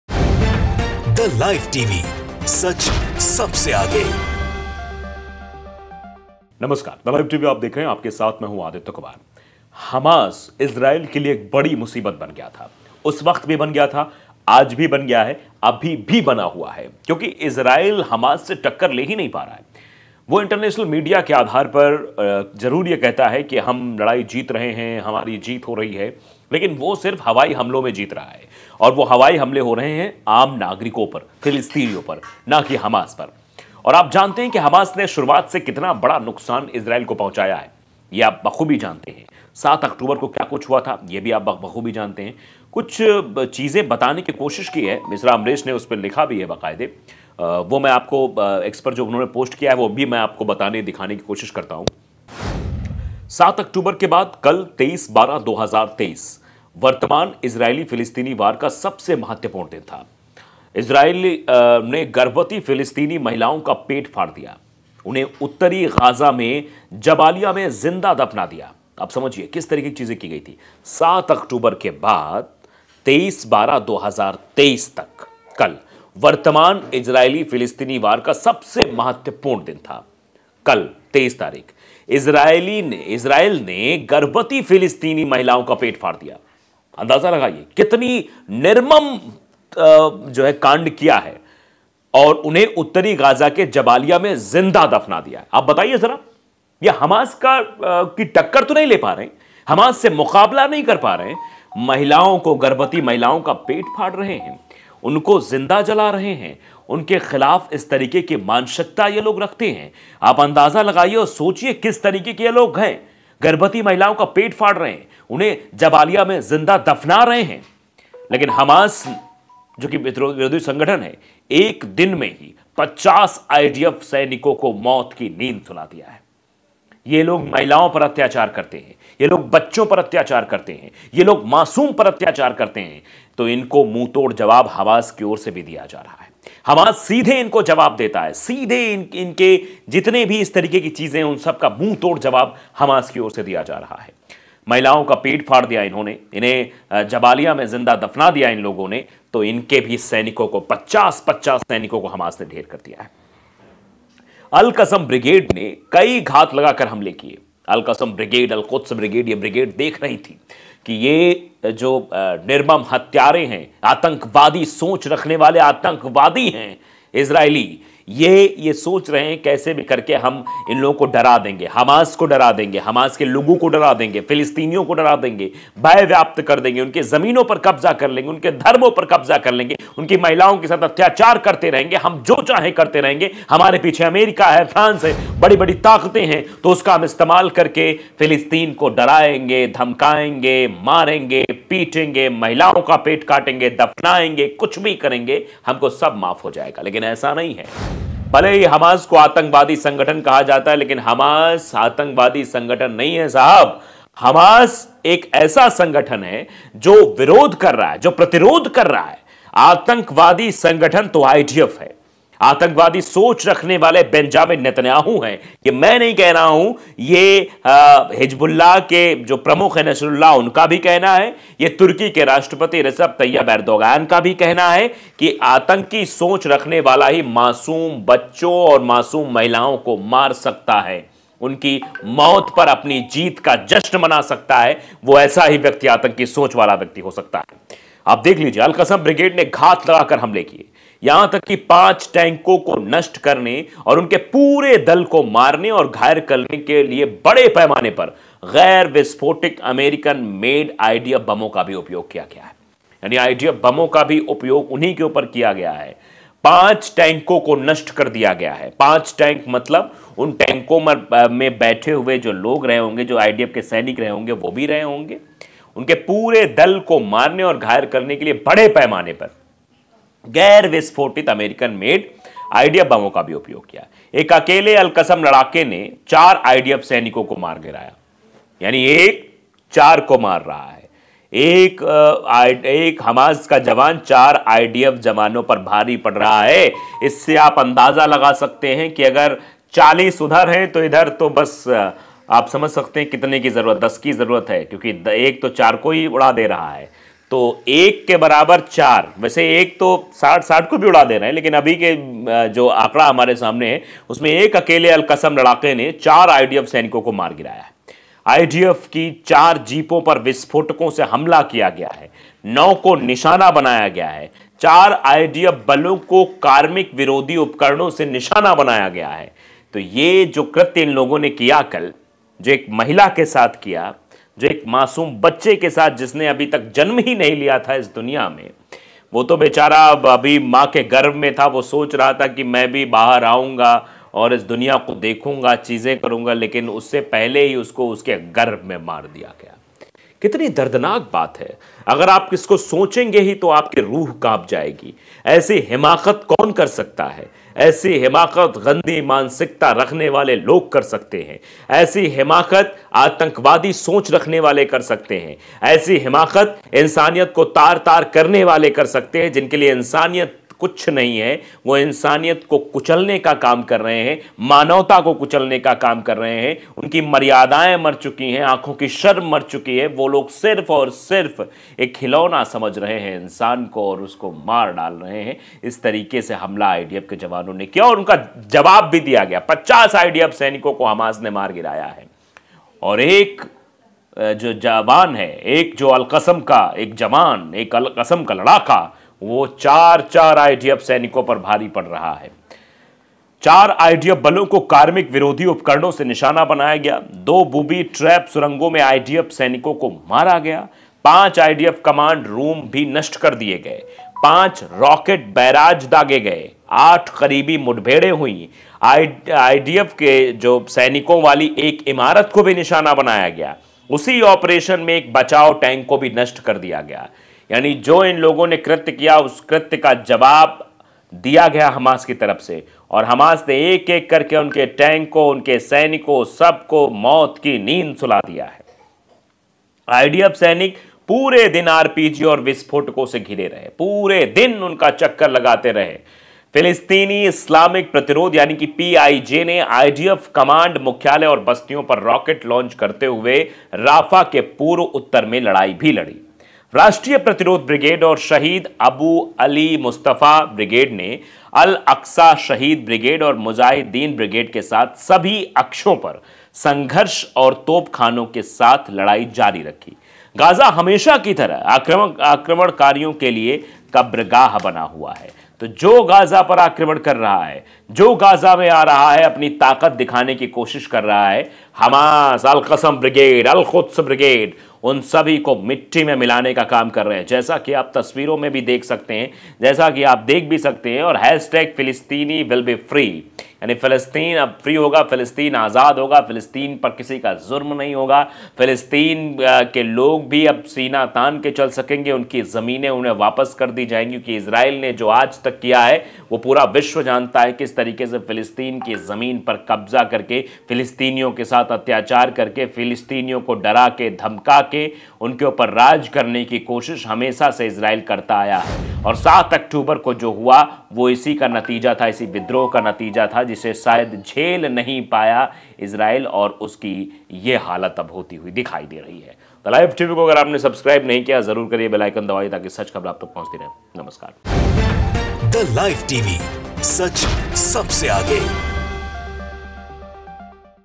इजराइली सेना के लिए हमास बना कयामत,50 सैनिक ढेर?
7 अक्टूबर के बाद, कल, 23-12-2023, वर्तमान IsraelPalestineWar का सबसे महत्वपूर्ण दिन था!
Israelis ने गर्भवती फिलिस्तीनी महिलाओं का पेट फाड़ दिया और उन्हें उत्तरी Gaza के जबालिया में जिंदा दफना दिया।
लेकिन Hamas/प्रतिरोध ने
एक ही दिन में 50 IDF सैनिकों को मार डाला/घायल कर दिया!
AlQassam ब्रिगेड ने कई घात लगाकर हमले किए, यहां तक कि 5 टैंकों को नष्ट करने और उनके पूरे दल को मारने और घायल करने के लिए बड़े पैमाने पर, गैर-विस्फोटित, American made, IDF बमों का भी उपयोग किया।
एक अकेले अल क़सम लड़ाके ने 4 IDF सैनिकों को मार गिराया।
IDF की 4 जीपों पर विस्फोटकों से हमला किया गया; 9 को निशाना बनाया गया.
4 आईडीएफ बलों को कार्मिक विरोधी उपकरणों से निशाना बनाया गया।
2 बूबी trap सुरंगों में IDF सैनिक मारे गए।
5 आईडीएफ कमांड रूम नष्ट कर दिए गए।
2 रॉकेट बैराज दागे।.
8 करीबी मुठभेड़ हुईं।
IDF सैनिकों वाली 1 इमारत को निशाना बनाया गया। उसी ऑपरेशन में एक बचाव टैंक नष्ट कर दिया गया।
IDF सैनिक पूरे दिन आरपीजी और विस्फोटकों से घिरे रहे।
फिलिस्तीनी इस्लामिक प्रतिरोध (पीआईजे) ने IDF कमांड मुख्यालय और बस्तियों पर रॉकेट लॉन्च करते हुए राफा के पूर्व और उत्तर में लड़ाई लड़ी।
राष्ट्रीय प्रतिरोध ब्रिगेड और शहीद अबू अली मुस्तफा ब्रिगेड ने अल-अक्सा शहीद ब्रिगेड और मुजाहिदीन ब्रिगेड के साथ सभी अक्षों पर संघर्ष और तोपखाने के साथ लड़ाई जारी रखी।

गाजा, हमेशा की तरह, आक्रमणकारियों के लिए कब्रगाह बना हुआ है।